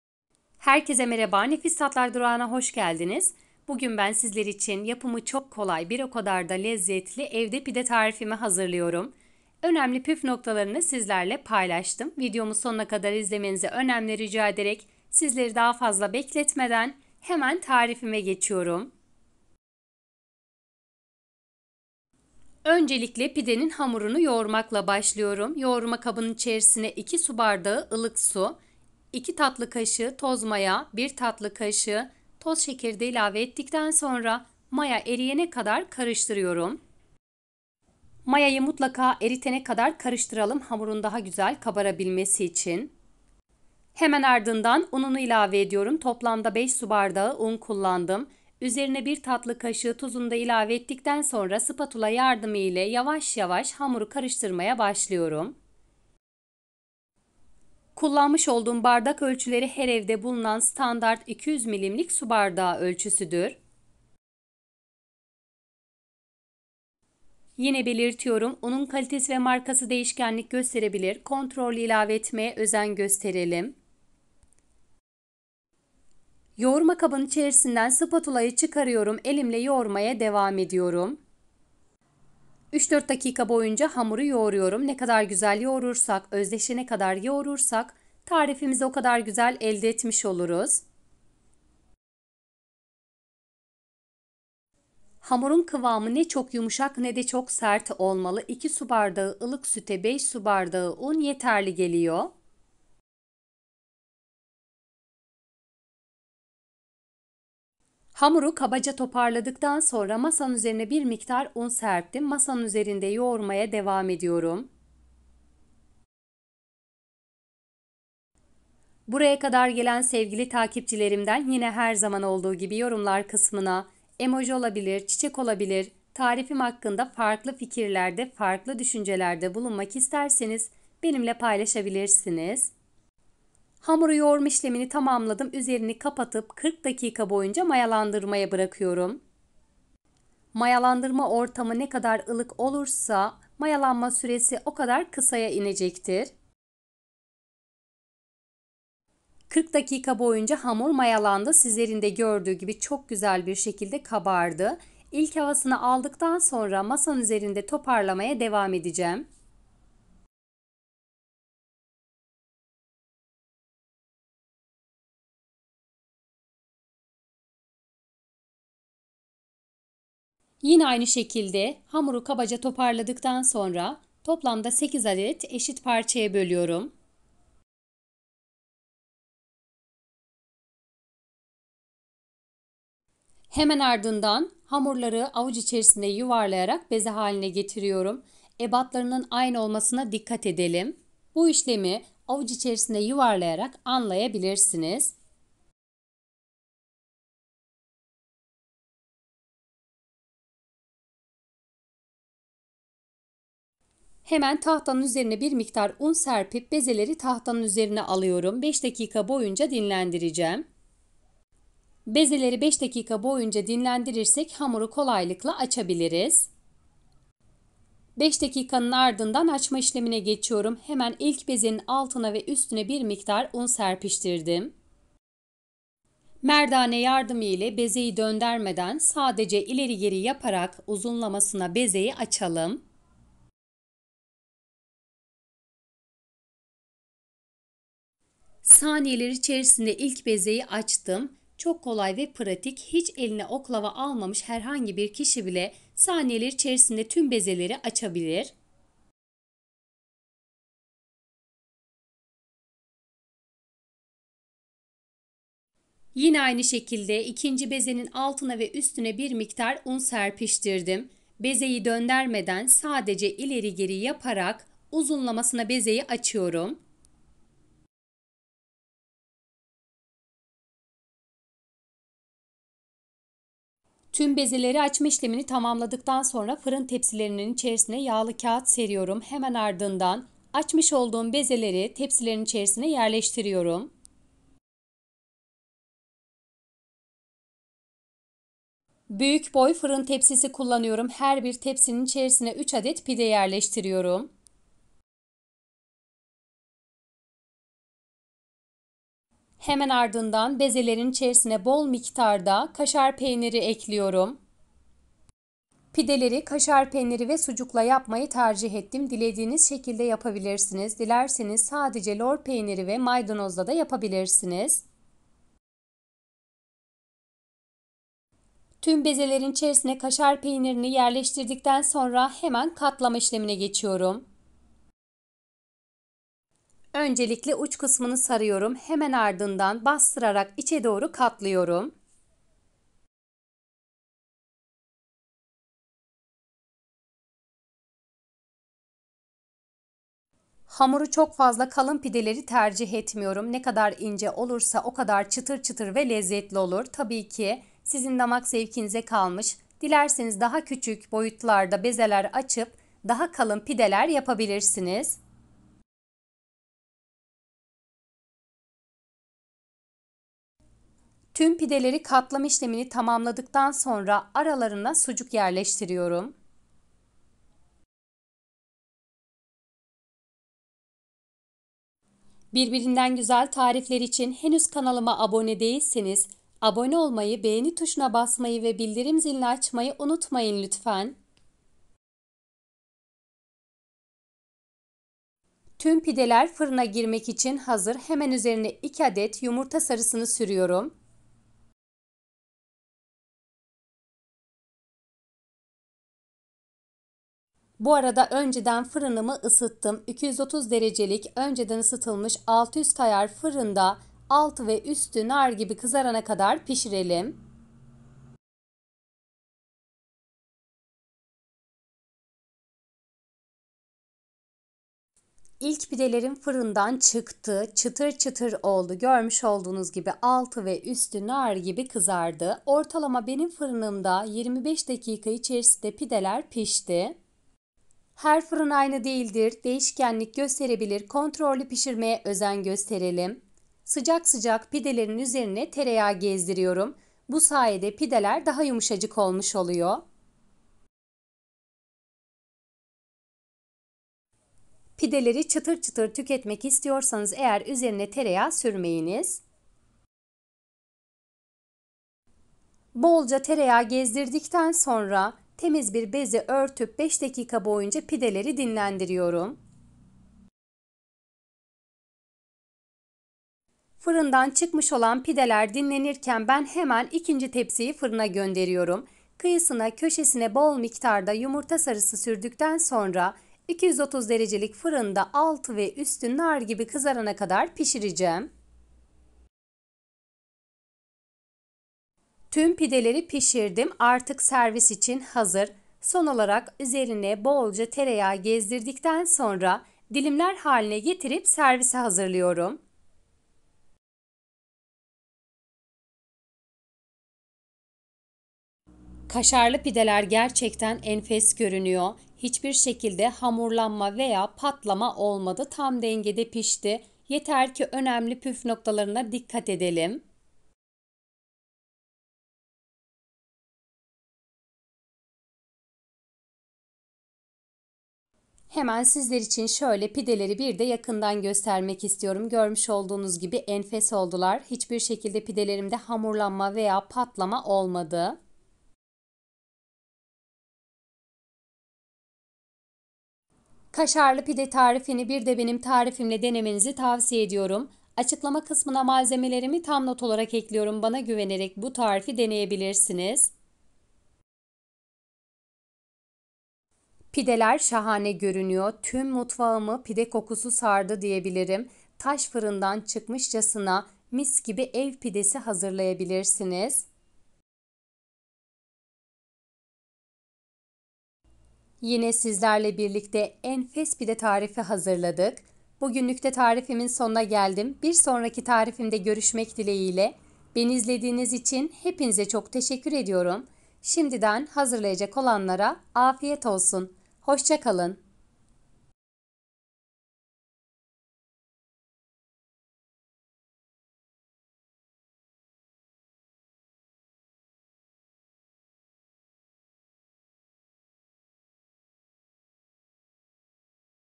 🔘Evde kaşarlı PİDE Yapımı [Kaşarlı PİDE nasıl yapılır?🔝Hamur işi/kolay pide tarifi.
Evde kaşarlı PİDE tarifi 👇👇
malzemeler 👇👇
hamuru için
2 su bardağı ılık su
5 su bardağı un
2 tatlı kaşığı toz maya
1 tatlı kaşığı tuz
1 tatlı kaşığı toz şeker

içerisine 👇👇
kaşar peyniri
sucuk

üzerine 👇👇
yumurta sarısı